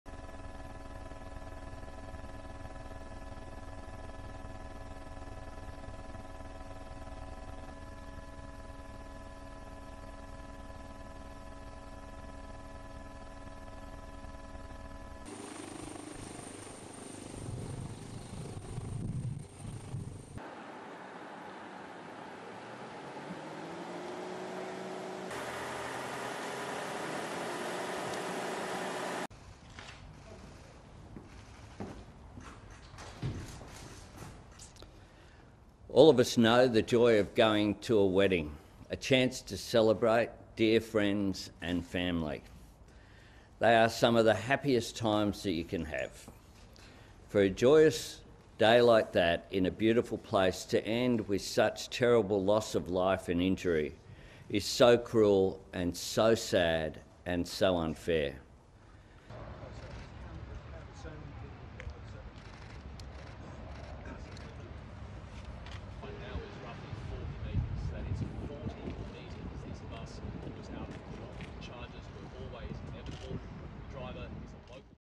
Wedding bus crashes in Australia
Bus carrying wedding guests in Australian wine region rolls over, killing 10 and injuring 25.